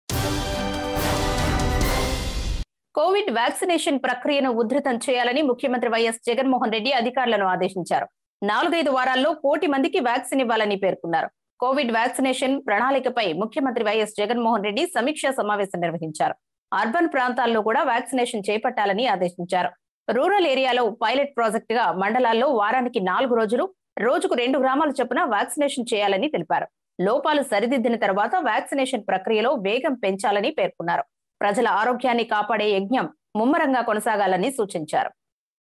Ys Jagan Review Meeting On Vaccination | పై జగన్ సీరియస్ | PDTV News
వ్యాక్సినేషన్ పై జగన్ సీరియస్